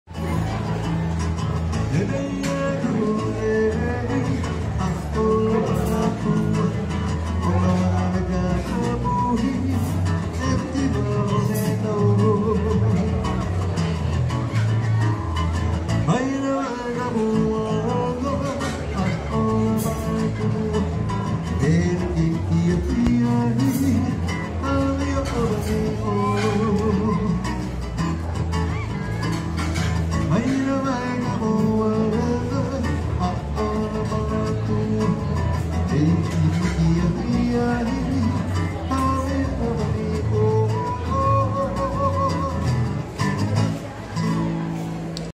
Taniec "Hula"
Taniec "Hula" przy plaży Waikiki na wyspie O'ahu (Hawaje)